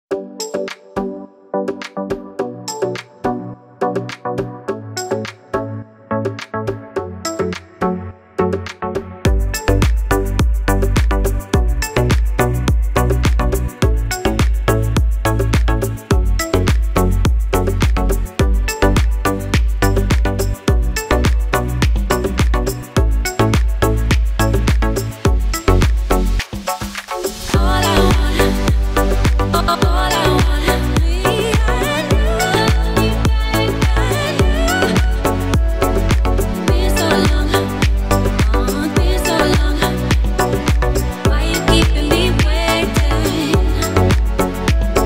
Campus Men Sandal [3k-908]
Hello Friends,
Kayam here in this short video quick look review of the Campus Men Sandal [3k-908] Under Rs.629 check it out links are down below.

Take care
Goodbye.